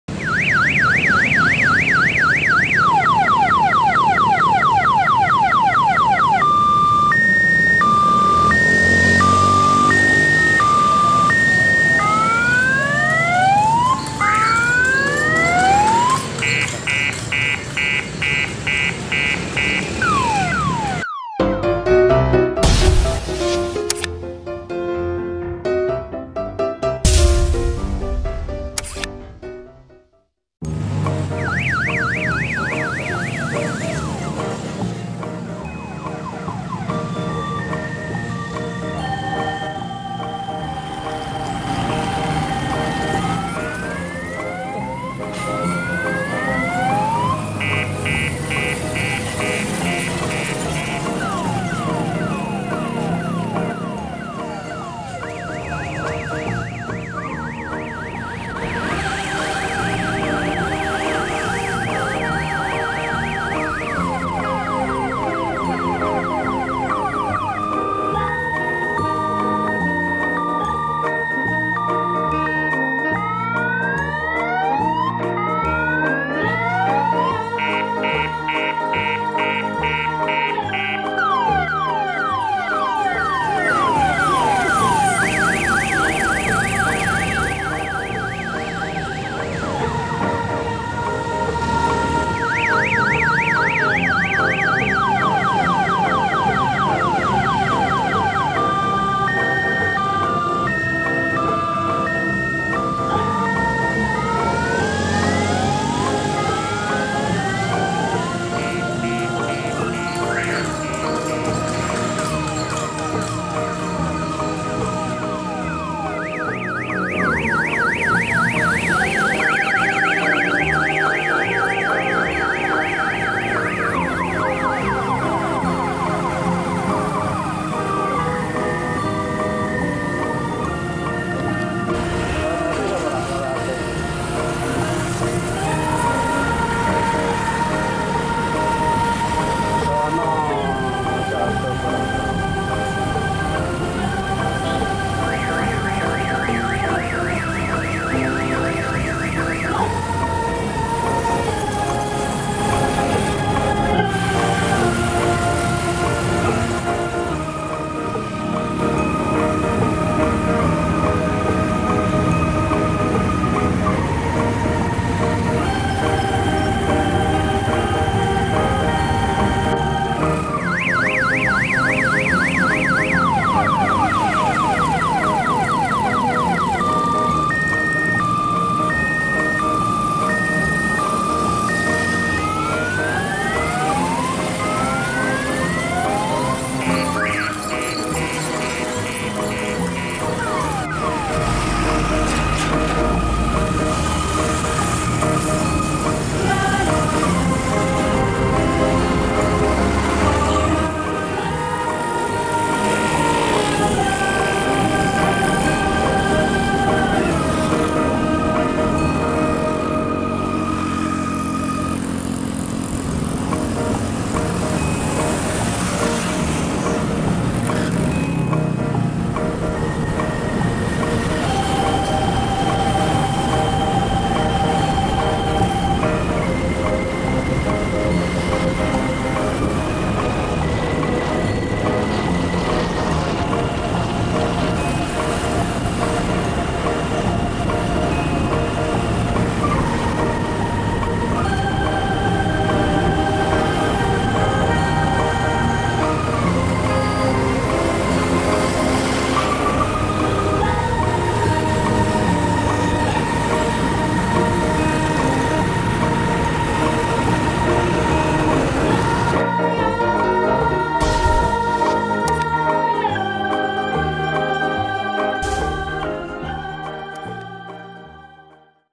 odong odong YELLOW THOMAS kereta wisata keliling kota CEPU
Odong odong or ondong ondong is a unique vehicle designation that is used as a means of transportation, both for transporting goods or transporting people. In today's world odong odong has become a vehicle for tourism in both rural and urban areas. Even on charters to transport family groups or certain groups. Currently, business competition in the world of tourist trains or mini trains is very tight. They odong odong entrepreneurs will make their vehicles more beautiful to attract their enthusiasts.